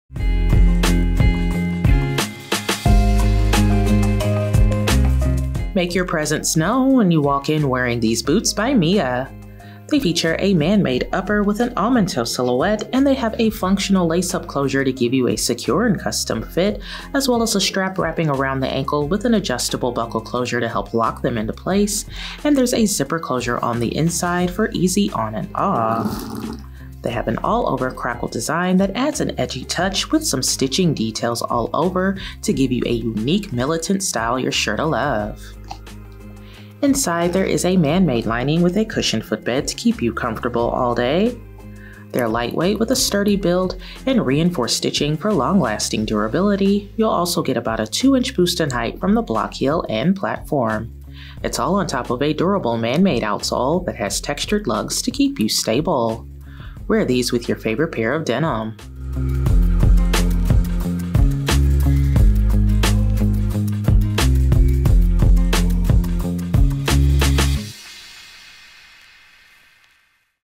MIA Charli SKU: 9794213
Mark your presence with your edgy style wherever you head wearing the  MIA™ Charli combat boots.
Man-made upper.
Man-made lining with a padded footbed for lasting comfort.
Adjustable lace-up front with a zippered side closure.
Wide strap detailing on the ankle with buckle closure.
Ankle-length construction.
Almond toe silhouette.
Pull tabs for easy on and off.
Contrasting stitched details on the midsole.
Stacked heel.
Durable man-made lug outsole.
Imported.